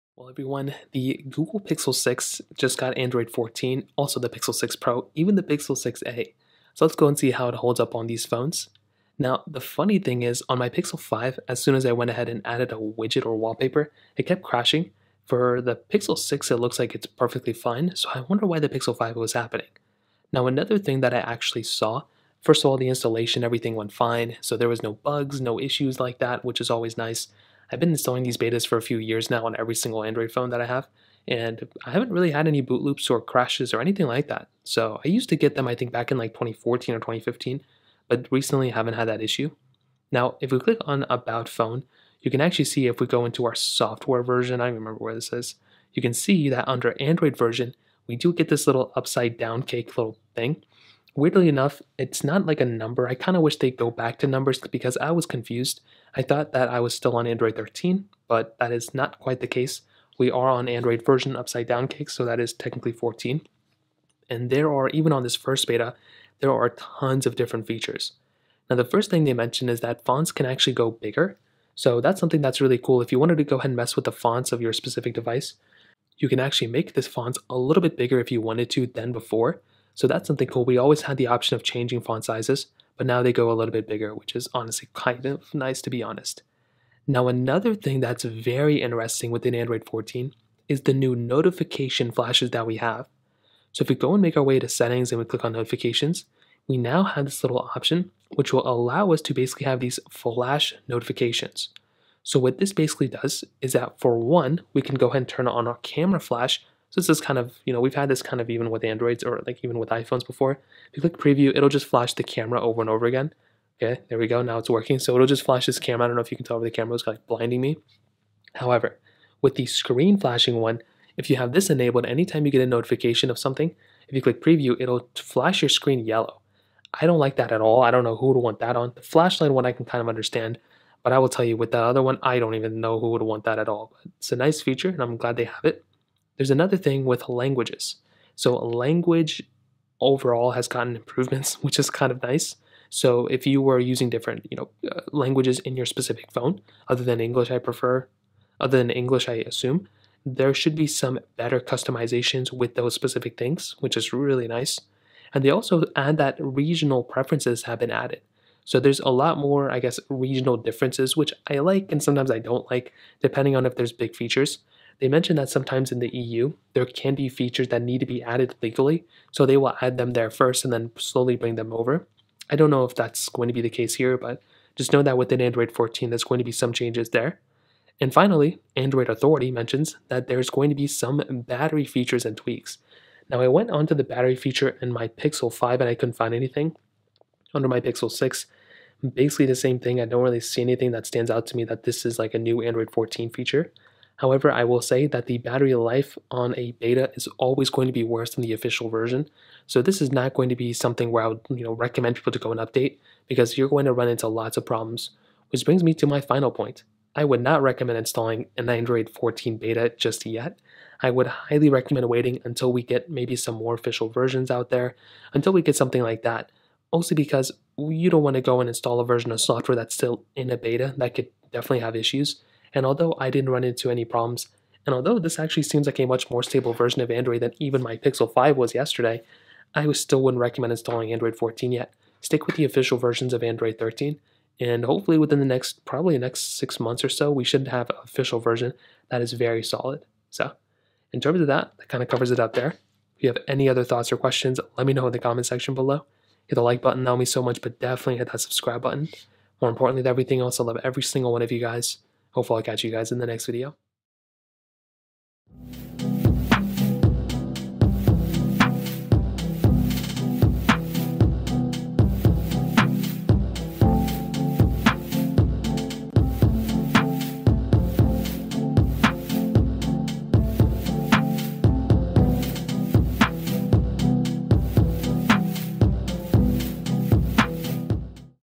Android 14 On Google Pixel 6/Google Pixel 6 Pro! (Review)
Let's take a look at the Android 14 On Google Pixel 6/Google Pixel 6 Pro! (Review)